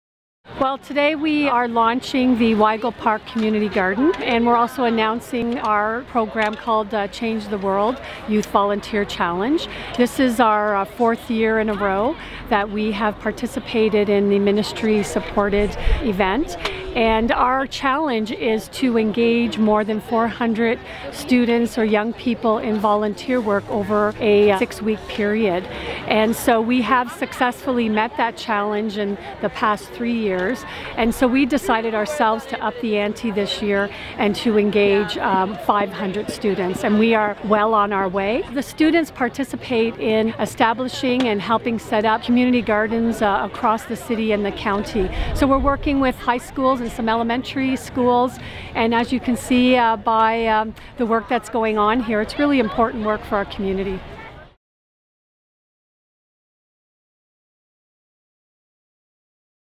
High school kids contribute to community garden
Lorraine Goddard, CEO of United Way/Centraide Windsor-Essex County, talks about the community gardens campaign.